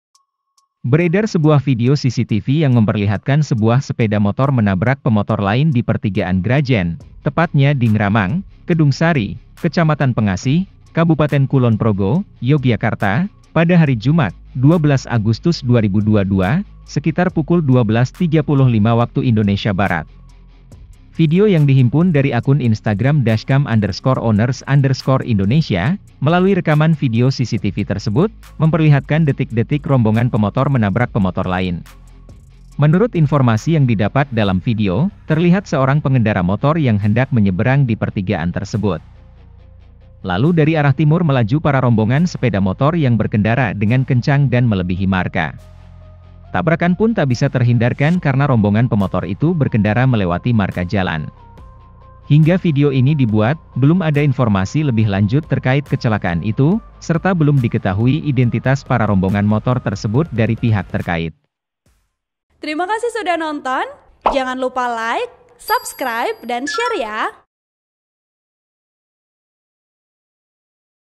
BERKENDARA UGAL - UGALAN ! Rombongan Pemotor Menabrak Pengendara Motor Lain yang Hendak Menyeberang
VP : Bimo Bagaswara

TRIBUNJATIM.COM - Beredar sebuah video CCTV yang memperlihatkan sebuah sepeda motor menabrak pemotor lain di Pertigaan Grajen, tepatnya di Ngramang, Kedungsari, Kecamatan Pengasih, Kabupaten Kulon Progo, Yogyakarta pada hari Jum'at (12/8/2022) Sekitar pukul 12.35 WIB.

Video yang dihimpun dari akun Instagram @dashcam_owners_indonesia melalui rekaman video CCTV tersebut memperlihatkan detik - detik rombongan pemotor menabrak pemotor lain.

Menurut informasi yang didapat dalam video, terlihat seorang pengendara motor yang hendak menyeberang di pertigaan tersebut. Lalu dari arah timur melaju para rombongan sepeda motor yang berkendara dengan kencang dan melebihi marka.

Tabrakanpun tak bisa terhindarkan karena rombongan pemotor itu berkendara melewati marka jalan. Hingga video ini dibuat, belum ada informasi lebih lanjut terkait kecelakaan itu, serta belum diketahui identitas para rombongan motor tersebut dari pihak terkait.